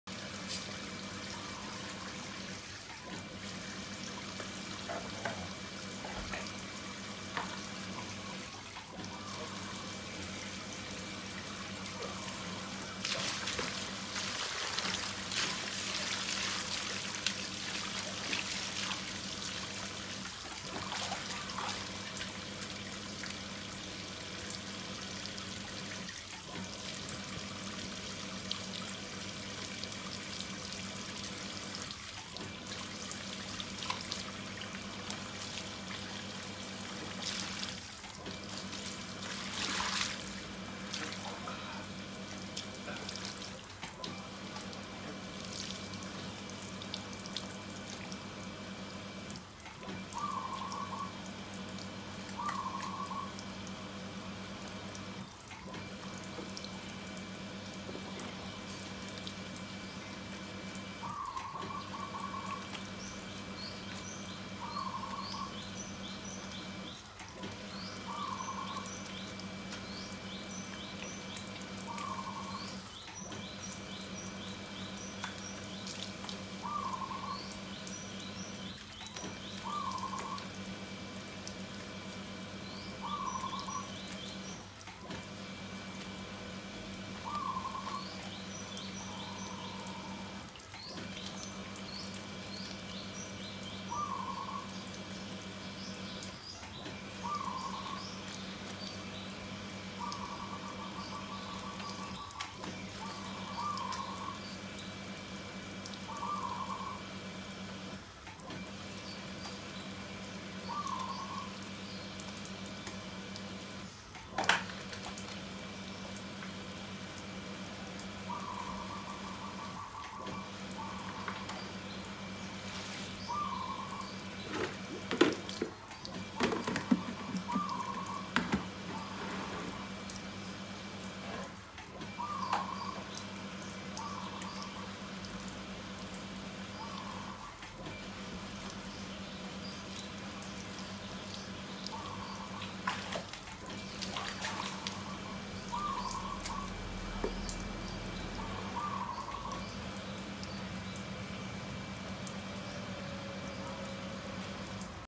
Golden Retriever Settles in Tub for 'Spa Day'
There comes a time when all creatures great and small deserve to kick back and relax, and for Biscuit the golden retriever, that means a “spa day” that includes a thorough cleaning from his owner.

Bangkok-based Thanankorn Pakdeeatsawasophon posted this footage to YouTube, where his Cookie Cute channel features numerous videos of his canine pets and their interactions with his family.

Six-year-old Biscuit is often featured in Pakdeeatsawasophon’s footage enjoying some creature comforts, with this latest video shared on February 22.